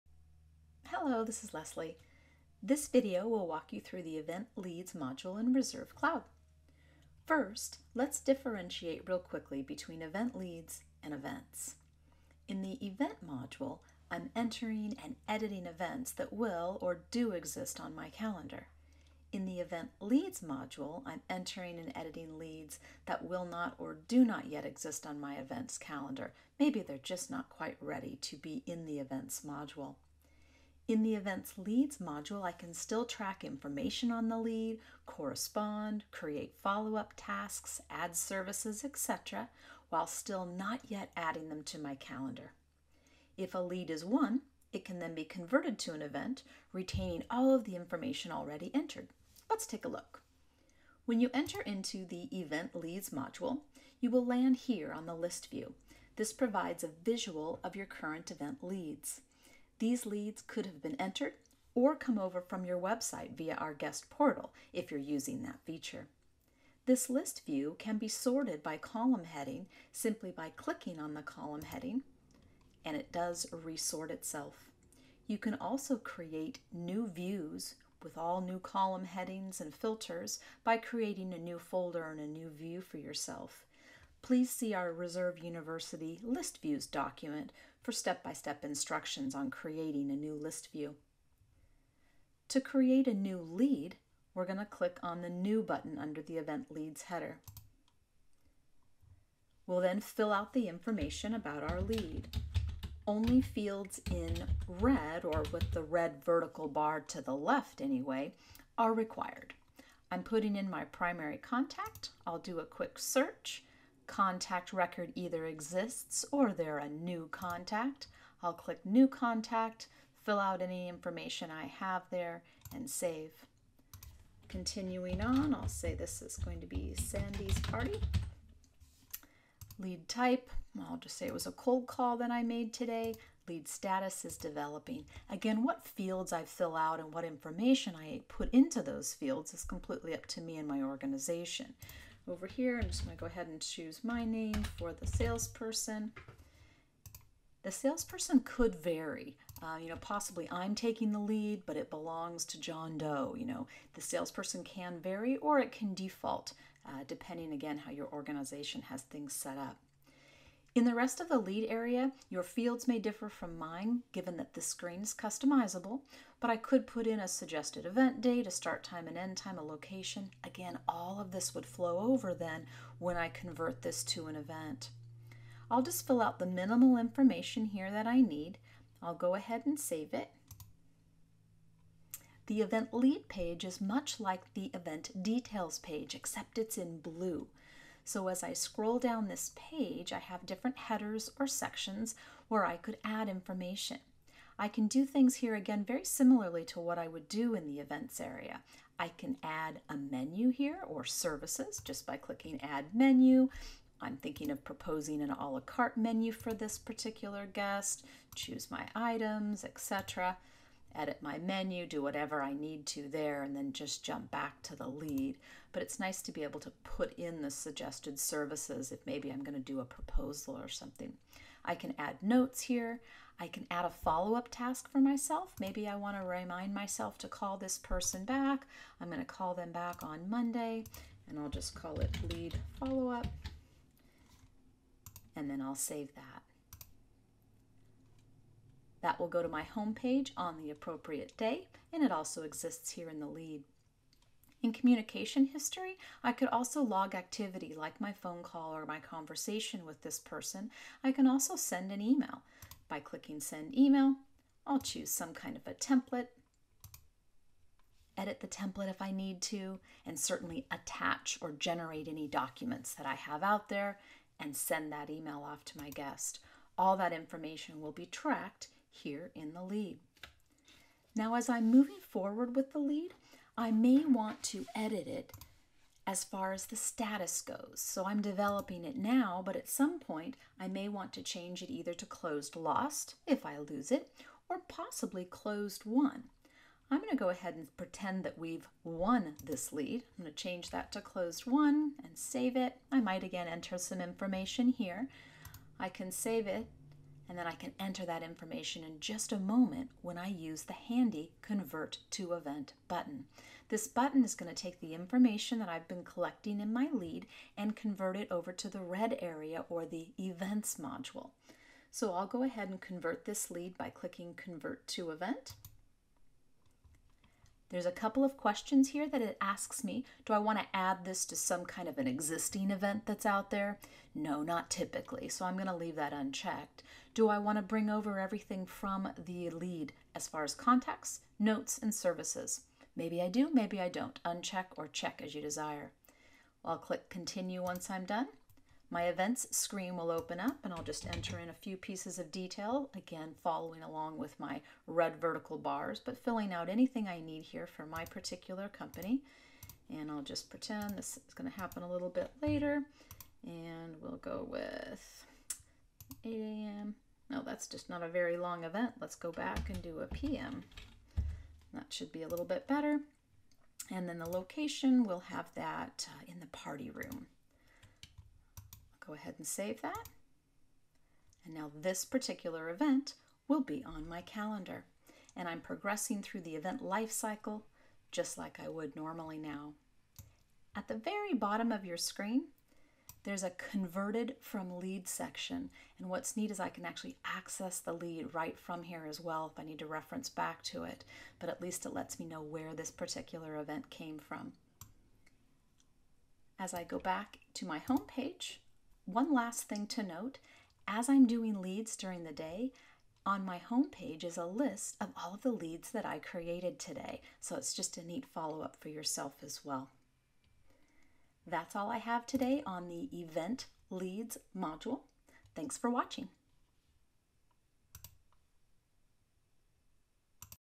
Getting Started: Event Leads
This video describes how enter a new event lead and convert it to an event.